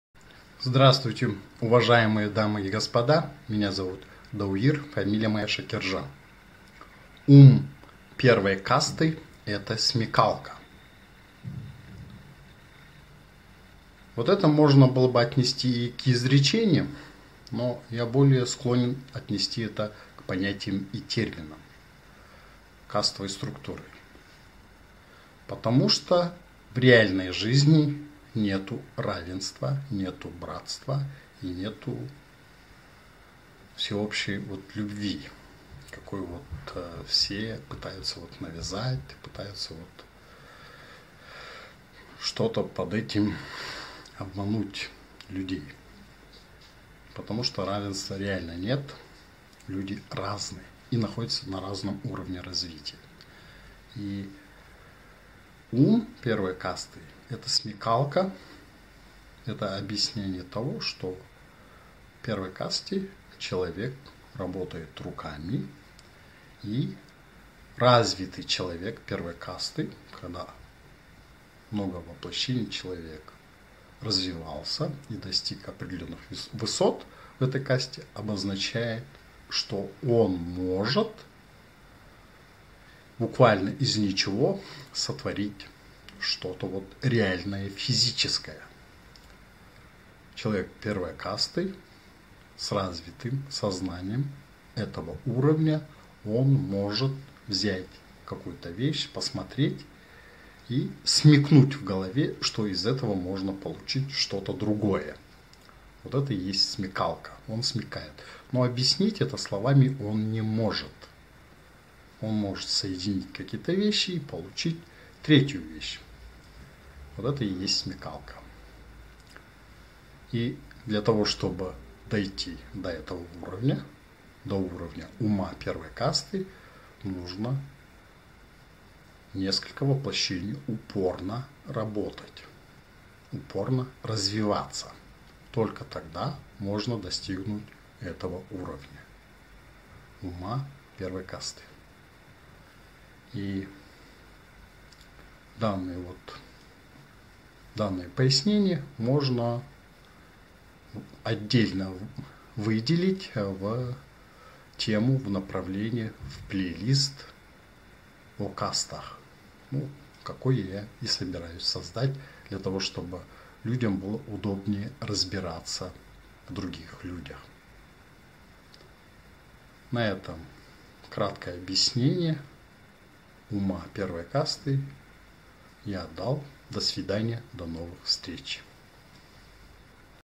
Ум первой касты - смекалка
Ум первой касты - смекалка. Это можно было бы назвать изречением Дауира Шакиржан, но я более склонен отнести это к понятиям и терминам по эзотерике.